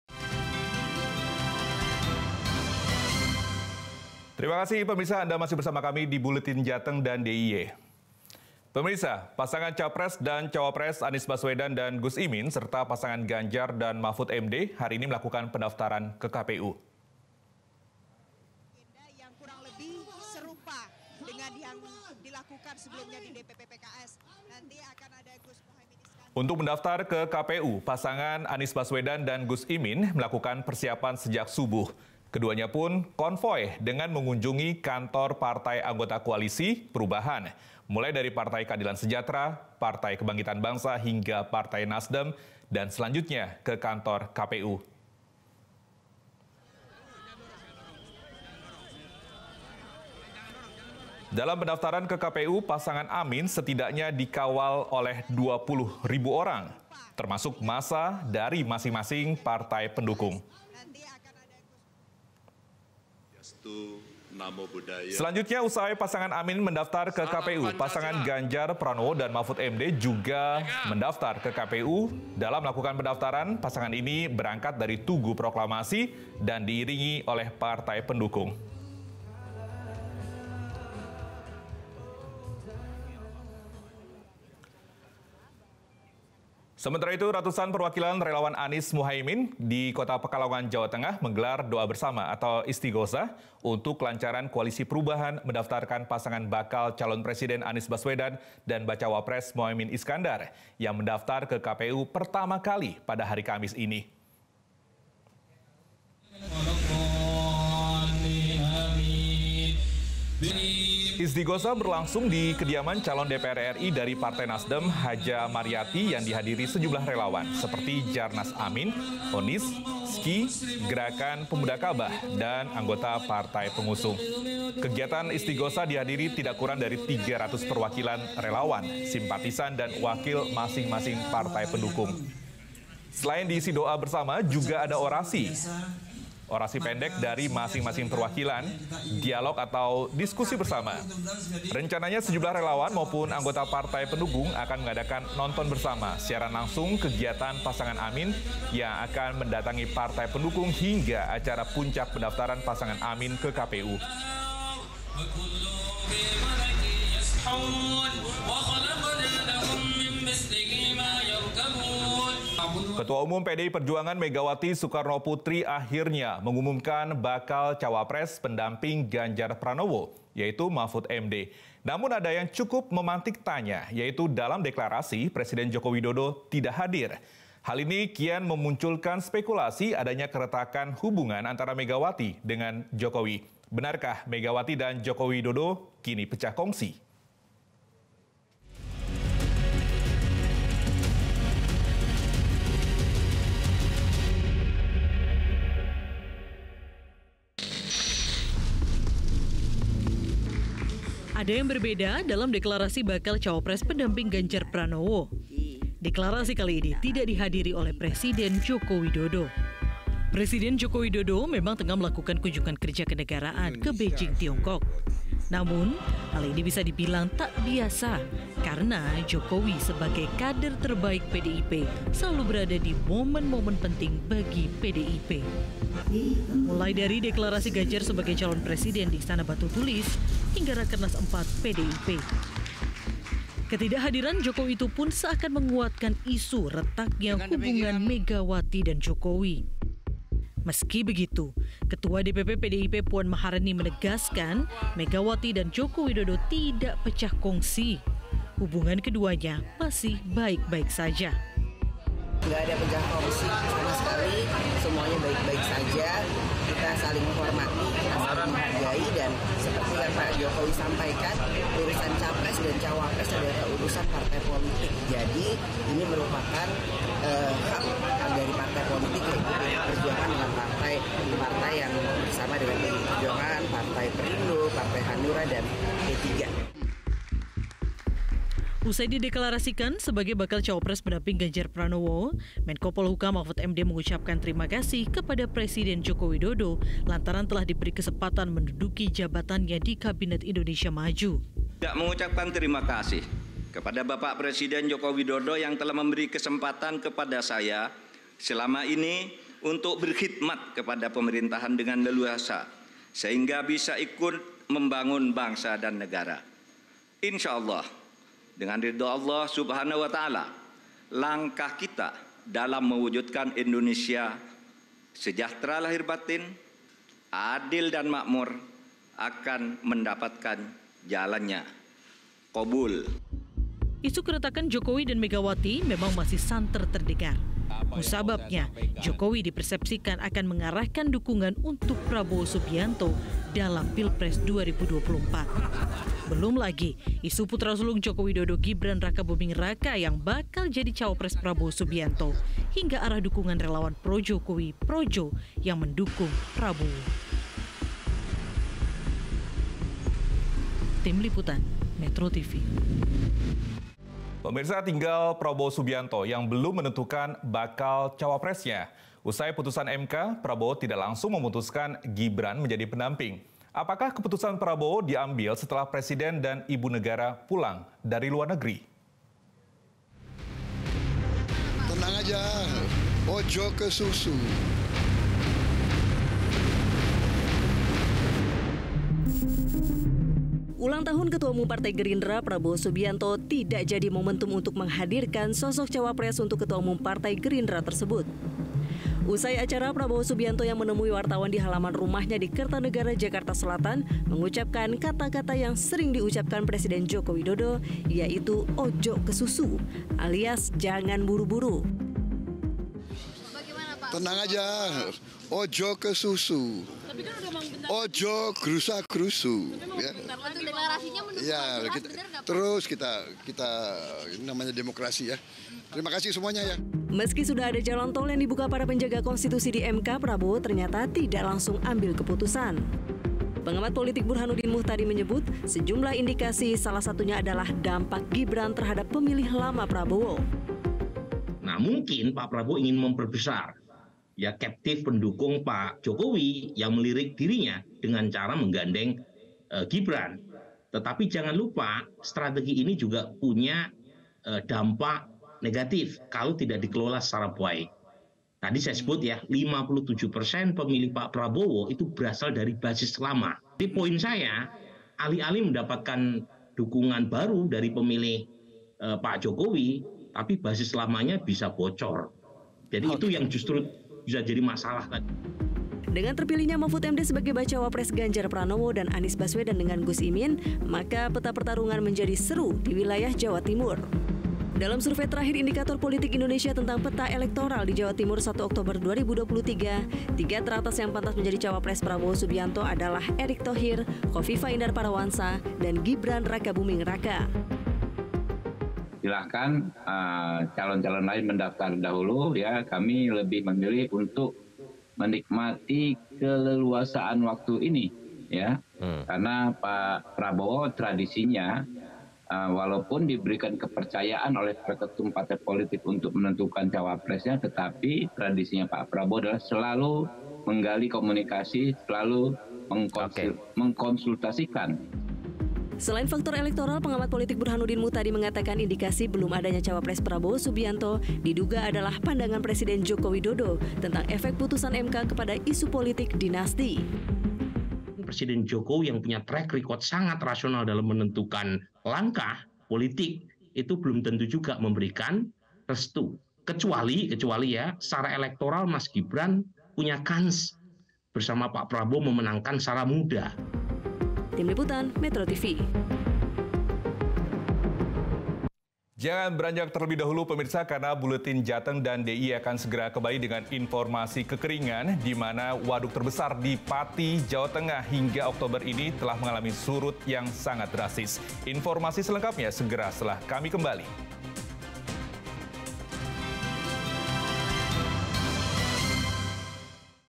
Capres & Cawapres Daftar ke KPU
METRO TV JATENG & DIY, Komisi Pemilihan Umum (KPU) telah menerima surat pemberitahuan soal pendaftaran dua pasang capres-cawapres, besok, Kamis (19/10/2023). AMIN menjadi pasangan capres yang pertama akan mendaftar ke KPU, kemudian disusul oleh pasangan Ganjar-Mahfud.